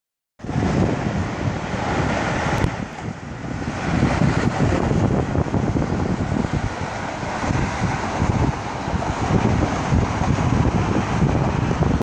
Red Dodge Chally SRT8 HEMI 20160528 075058
Riverside expressway accelerating HEMI V-8 Dodge Challenger SRT8!